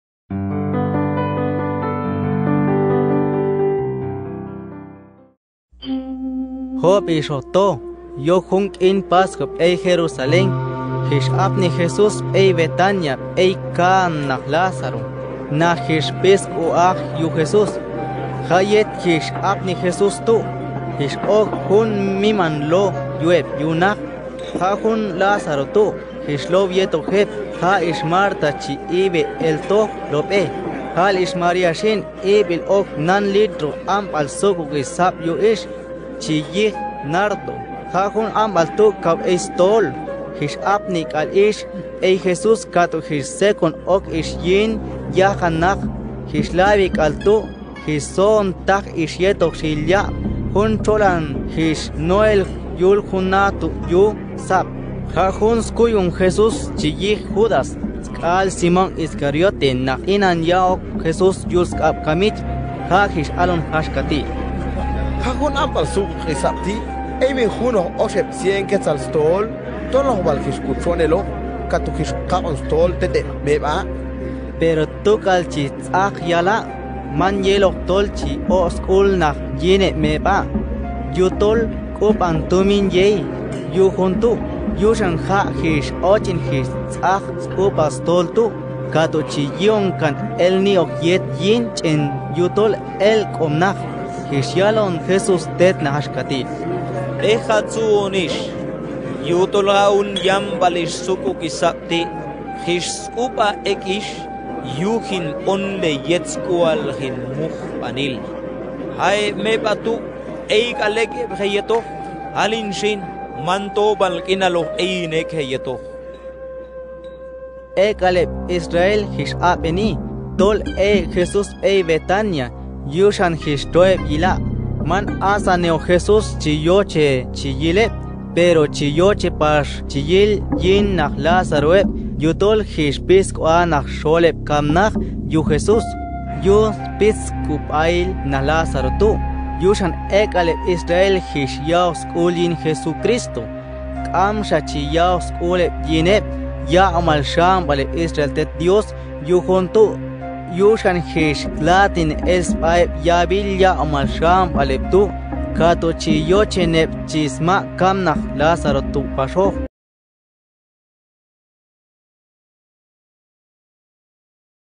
Akateko - John 12:1-11 - Mary anoints Jesus [knj]
Scripture Videos from the Gospel of John in Akateko of Guatemala and Mexico. [knj]